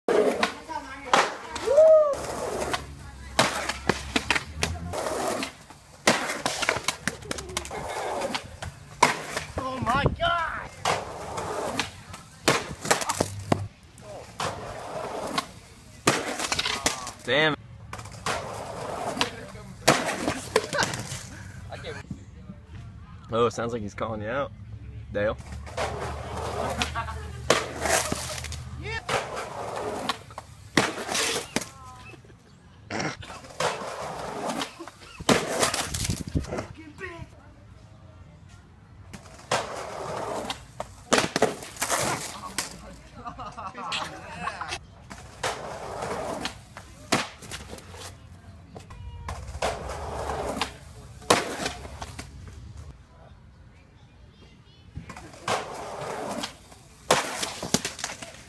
Mex getting
Mex trying to Ollie down band stand, and over the first crack. It was huge.